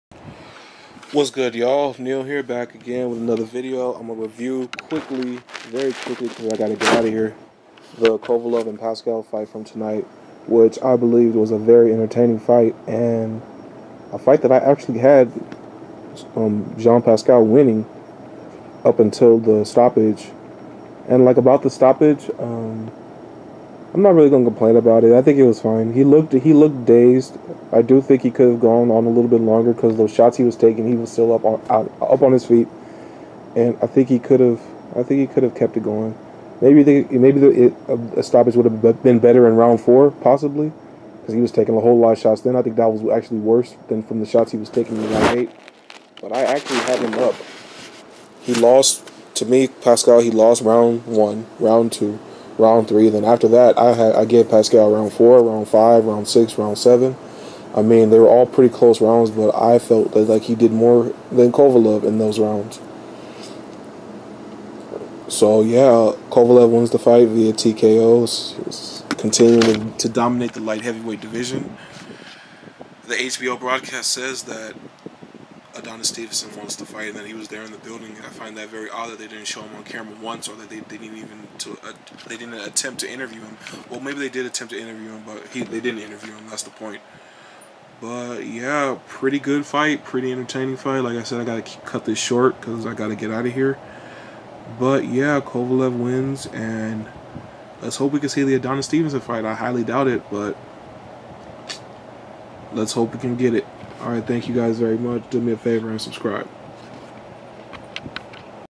HBO Sergey Kovalev vs Jean Pascal Post Fight Review
Quick post fight review of Sergey Kovalev vs Jean Pascal.